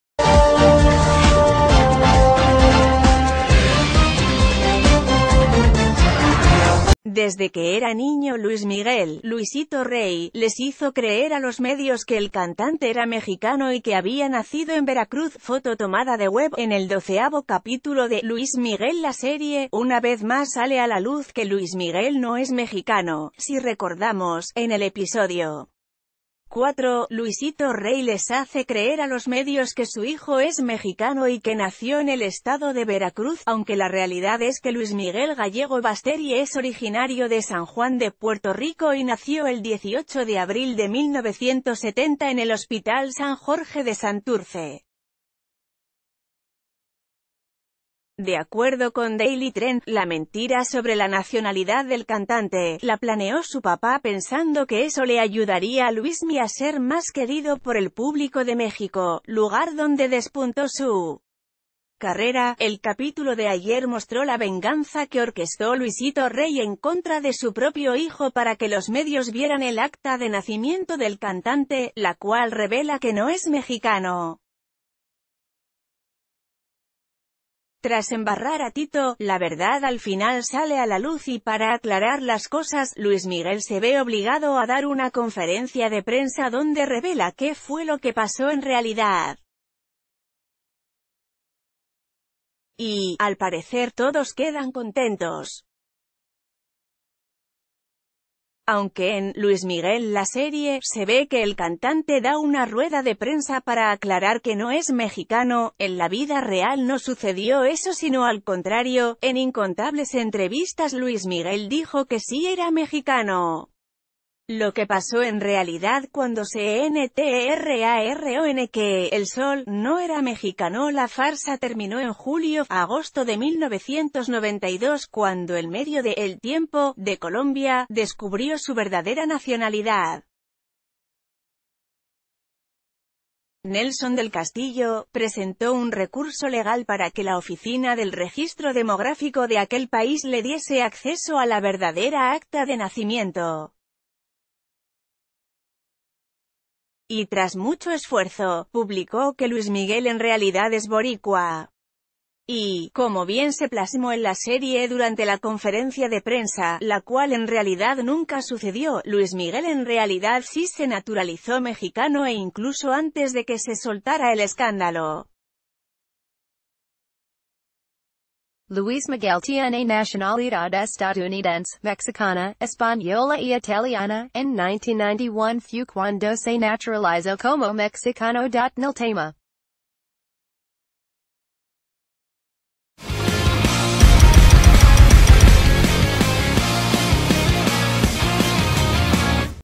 Así reveló Luis Miguel que era mexicano y no fue en una rueda de prensa
Desde que era niño Luis Miguel, Luisito Rey, les hizo creer a los medios que el cantante era mexicano y que había nacido en Veracruz (FOTO TOMADA DE WEB)En el doceavo capítulo de "Luis Miguel La Serie", una vez más sale a la luz que Luis Miguel no es mexicano; si recordamos, en el episodio cuatro, L...